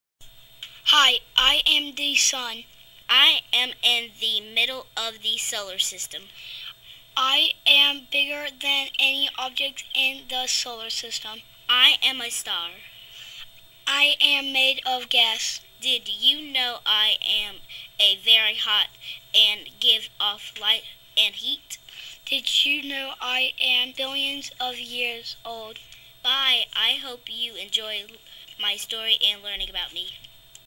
The Sun by London and Ryker
This is a space research project made by students in Mrs. Reid's 1st grade class.